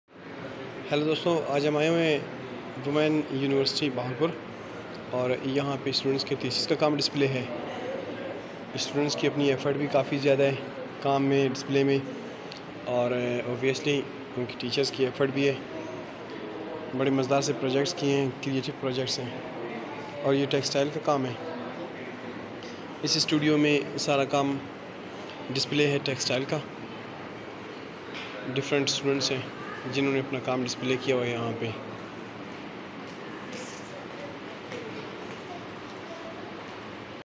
Women University | Bahawalpur | Exhibition
Thesis Painting, Textile Miniature Painting Exhibition at Women Sadiq University Bahawalpur 2021
@Zakir Studio